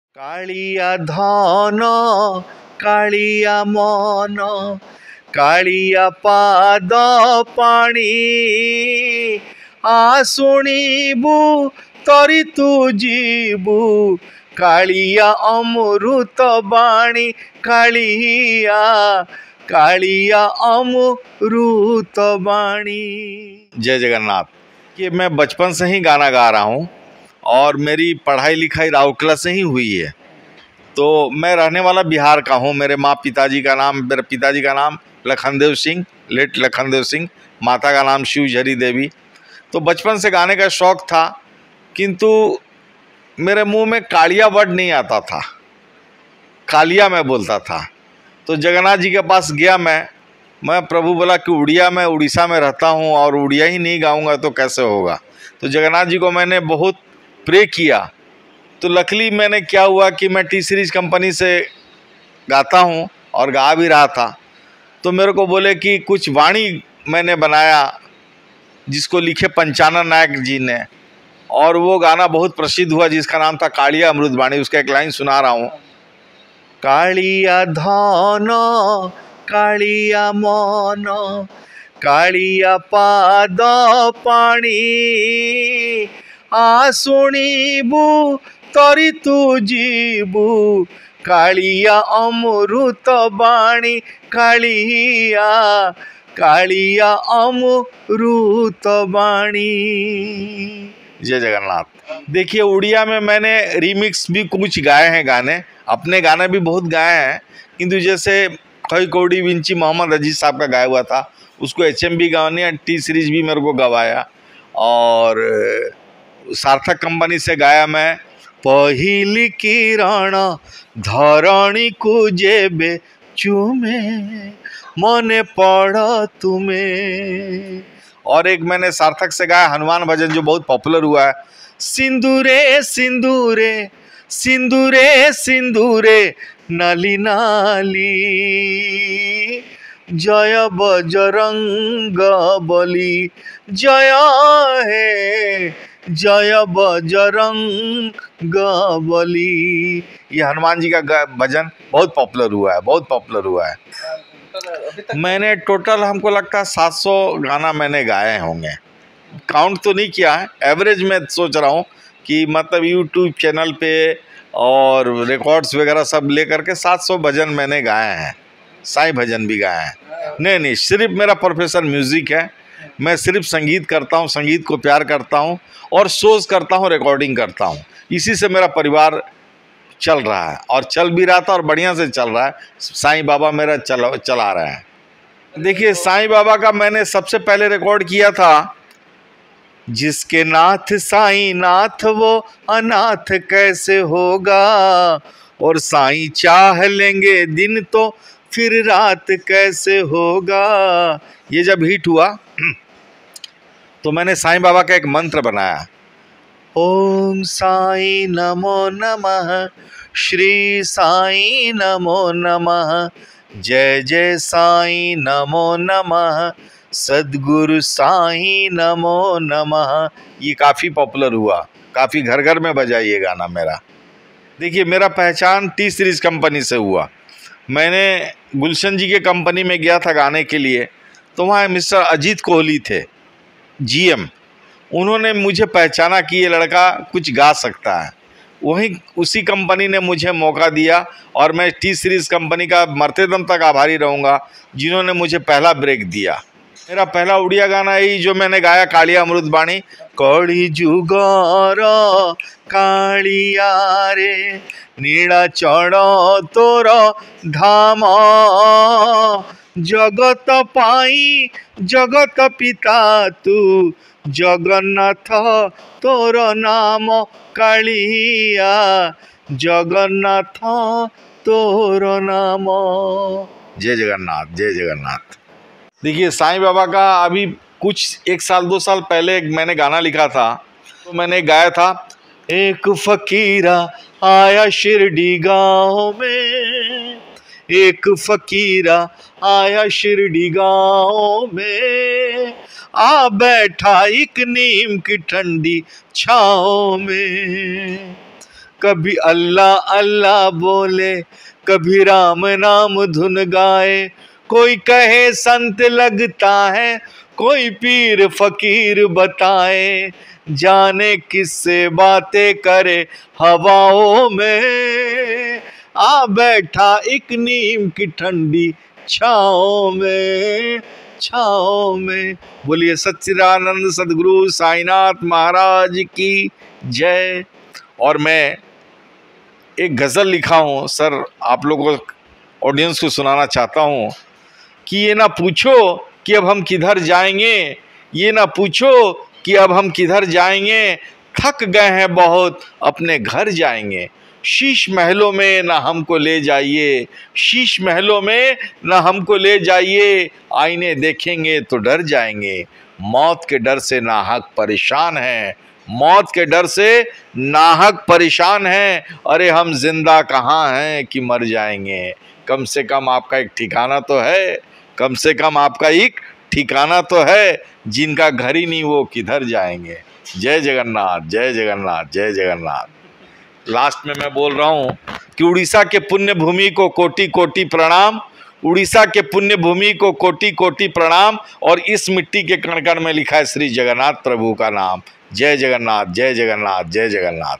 ଅଣଓଡ଼ିଆ ଭଜନ ଗାୟକ ଅନିଲ ବାୱରା, ଗାଇଲେଣି ୭୦୦ରୁ ଊର୍ଦ୍ଧ୍ବ ଭଜନ || JAGANNATH BHAJANA || ETV Bharat Odisha
ଜନ୍ମ ବିହାରରେ । ହେଲେ ଅଣ ଓଡ଼ିଆ ହୋଇ ମଧ୍ୟ ପ୍ରଭୁ ଜଗନ୍ନାଥଙ୍କ ପାଇଁ ଭଜନ ଗାଇ ସାଉଁଟିଛନ୍ତି ପ୍ରଶଂସା ।  ସାରା ଦେଶରେ ନିଜର ଭଜନକୁ ନେଇ ବହୁ ପ୍ରଶଂସିତ ହୋଇଛନ୍ତି ସେ । ଦିନେ ତାଙ୍କ ପାଟିରେ 'କାଳିଆ' ଶବ୍ଦ ପଶୁନଥିଲା । ଏଥିଲାଗି ସେ ବ୍ୟସ୍ତବିବ୍ରତ ହୋଇ କାଳିଆ ଦୁଆରେ ପହଞ୍ଚିଥିଲେ । ଆଉ କାଳିଆକୁ ପ୍ରାର୍ଥନା କରିଥିଲେ । ଏହାପରଠୁ ସେ ଆଉ ପଛକୁ ଚାହିଁନାହାନ୍ତି । ଆଜି ପର୍ଯ୍ୟନ୍ତ ହିନ୍ଦୀ, ଓଡ଼ିଆ ଆଦି ଭାଷାର ପ୍ରାୟ ୭୦୦ରୁ ଊର୍ଦ୍ଧ୍ବ ଭଜନ ଗାଇଲେଣି । ସଂଗୀତ ଜୀବନର ପ୍ରାରମ୍ଭରେ ସେ କରିଥିବା ସଂଘର୍ଷ ସମ୍ପର୍କରେ ଇଟିଭି ଭାରତ ସହିତ ଆଲୋଚନା କରିଛନ୍ତି । ଦେଖନ୍ତୁ ସମ୍ପୂର୍ଣ୍ଣ ସାକ୍ଷାତକାର